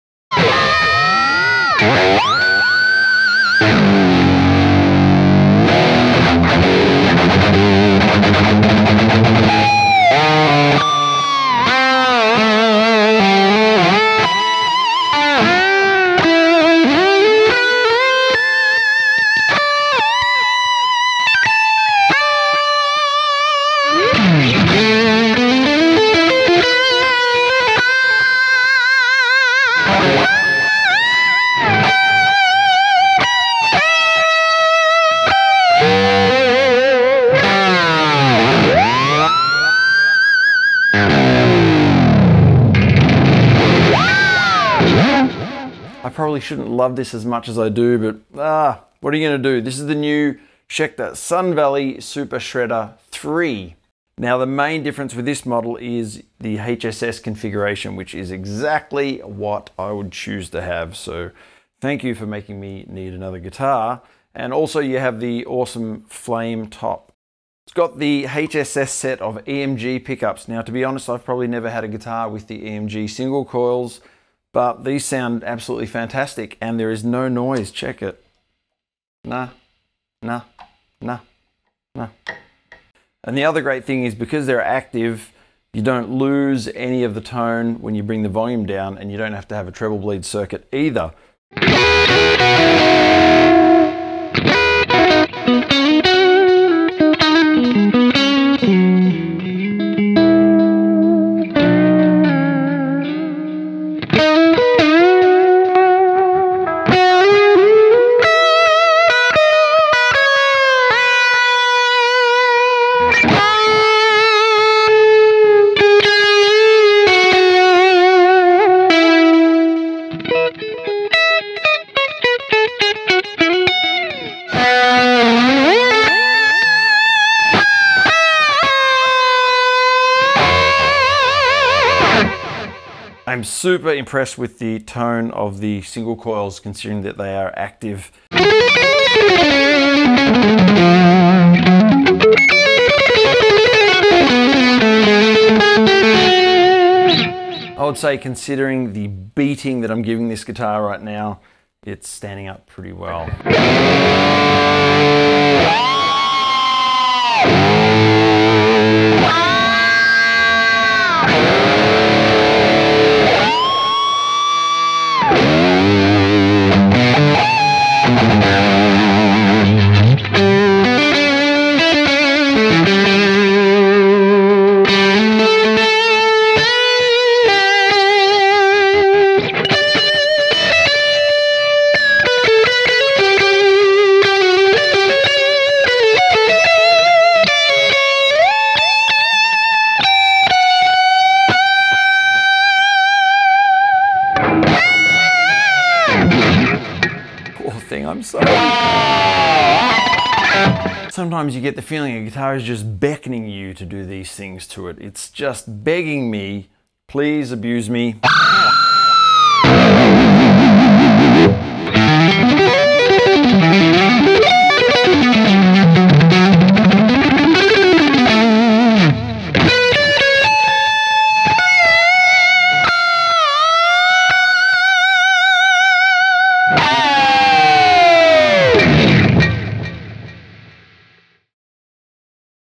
Schecter Sun Valley Super Shredder 3 with EMG RetroActive pickups
A hell of a lot of fun!  This guitar screams and then screams some more. I was pleasantly surprised with the tone of the new EMG Retroactive single coils.